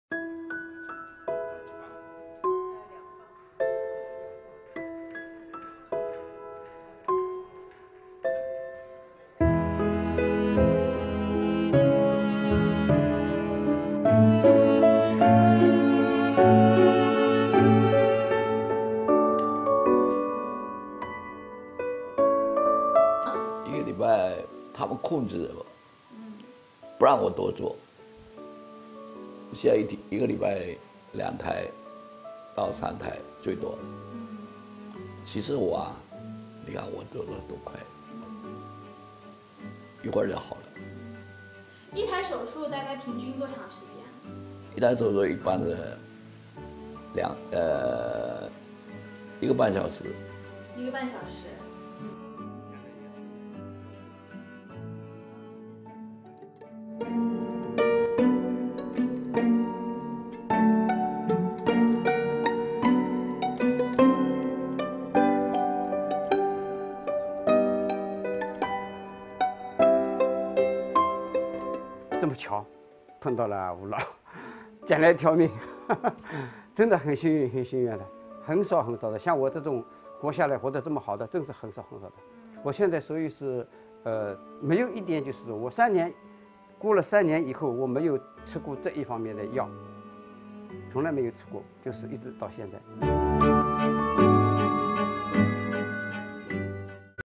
Hale and hearty: 96-year-old surgeon still works at operating table
Why a 96-year-old surgeon still works at operating table? Wu Mengchao has been looked at as a pioneer in the field of hepatobiliary surgery in China. Despite his age, Wu still does surgeries every week.